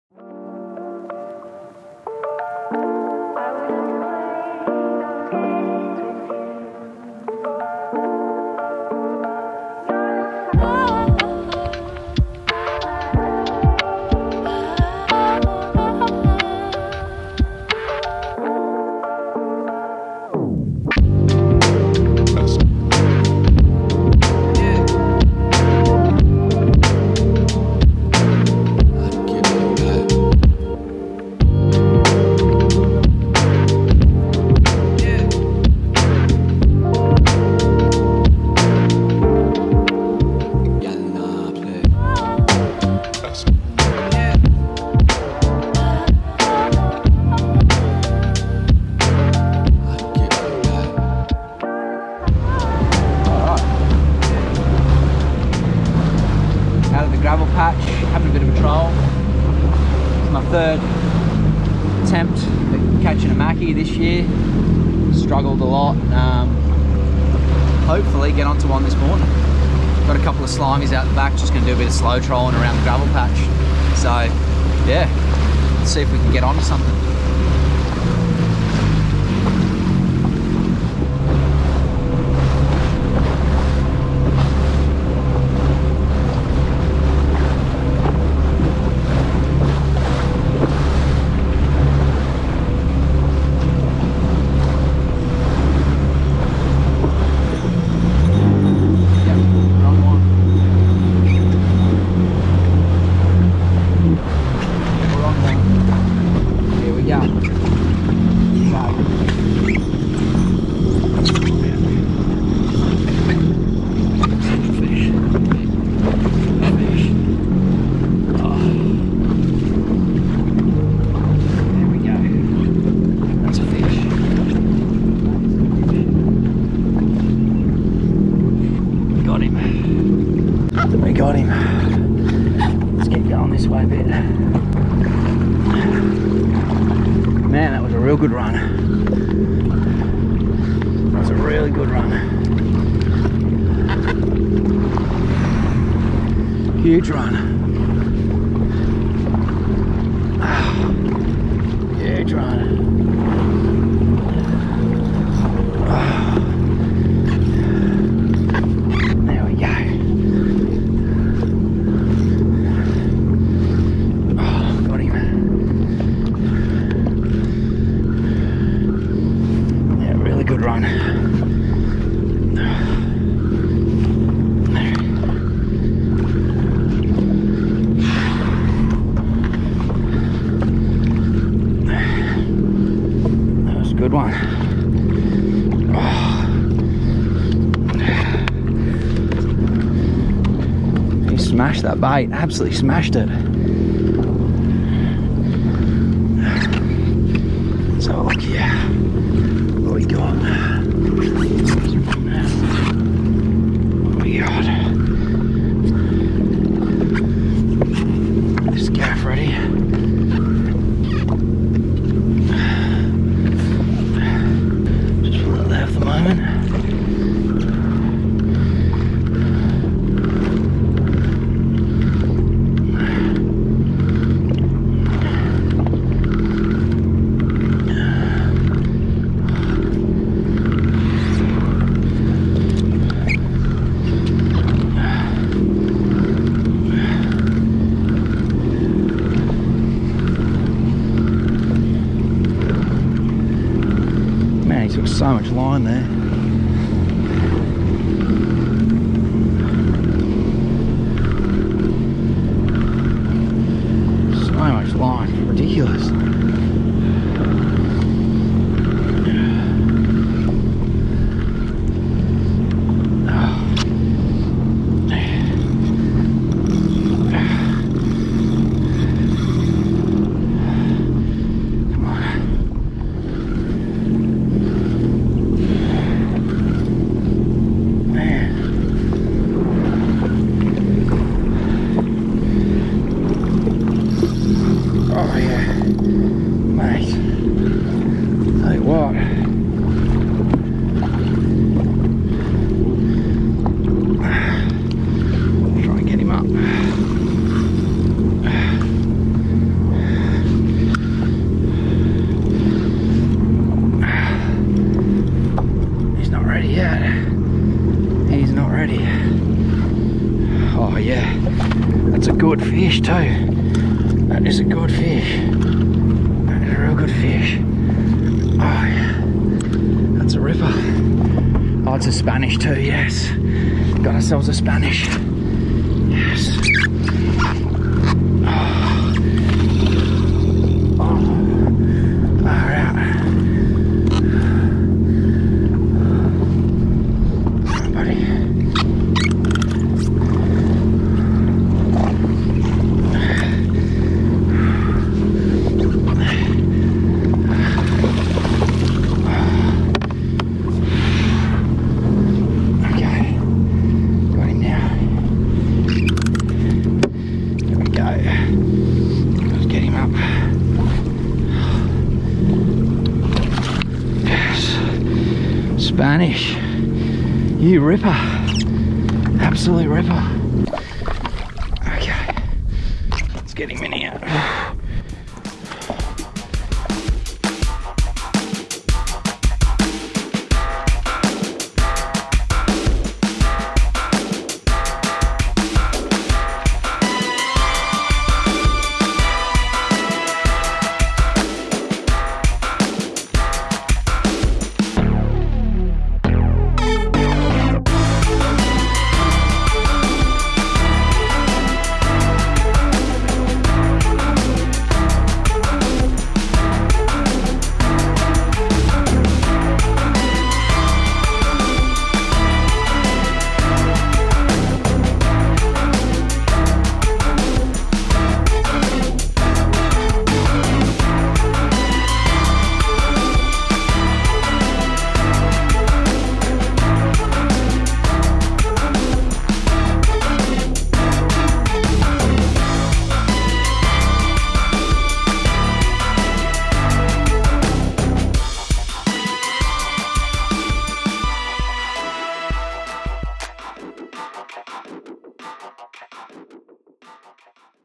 Finally , I did it !!! Gold Coast Spanish Mackerel | Jet Ski Fishing Gold Coast
3rd times a charm, finally cracked the Spanish code !  Cheers Legends !

Rods - Oceans Legacy Deep Element 4+PE – Penn Fathom 15 Lever Drag, 60lb Grappler Braid, Platypus 60lb Hard Armor Tough Leader

Samaki Vanquish  Pe4 - Penn Slammer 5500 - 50lb Grappler Barid, Platypus 60lb Hard Armor Tough Leader

Camera's - GoPro Hero 8 & 9